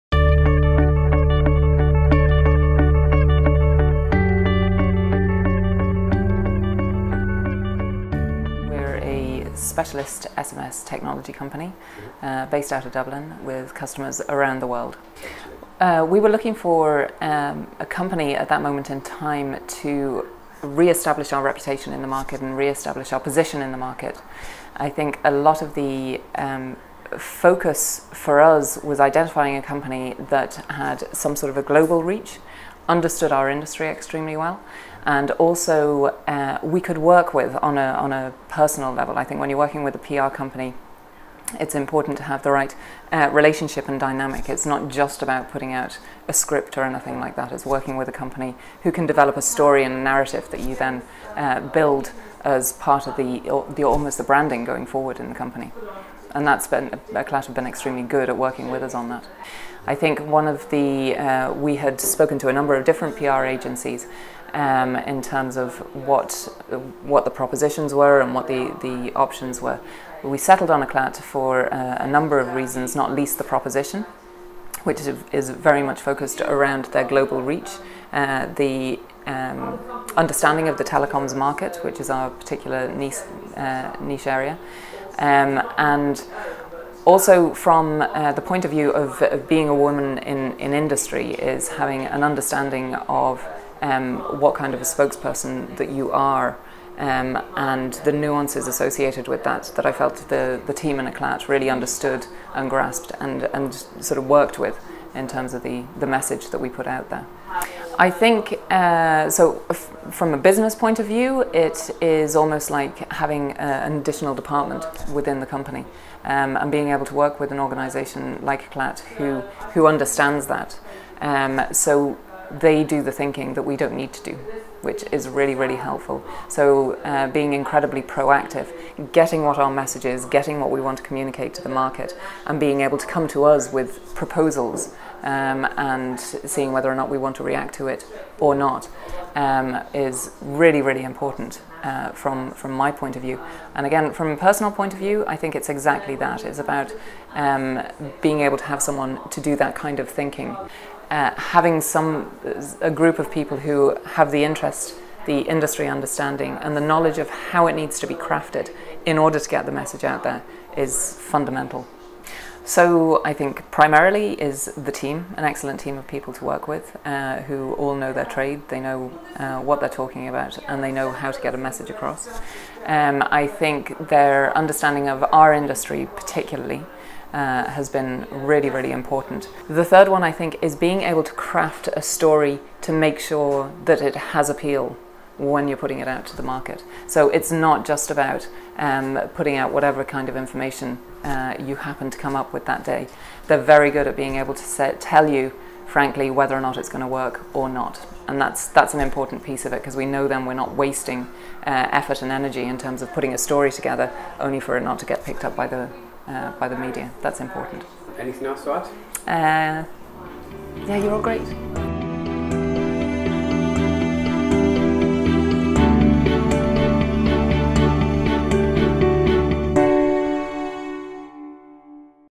Tech PR agency eclat Marketing client reference: Louise O'Sullivan, Anam Technologies
Louise O'Sullivan, CEO of Anam Technologies, discusses why she appointed eclat Marketing to conduct PR and AR acitivites and the benefits of appointing a Tech PR agency.